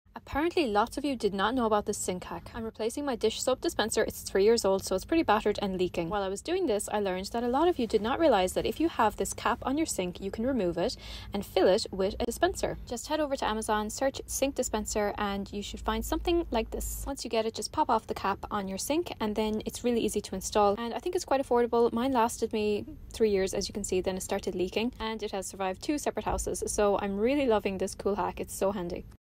Amazon Kitchen Sink Dispenser Hack | Kitchen Organisation | Home DIY
Try this Amazon Soap Dispenser Hack for your Kitchen Sink and have one less item cluttering you counter top.

After 3 years of using this dispenser I approve this hack!

Follow Home DIY Diary on Instagram for daily Home DIY updates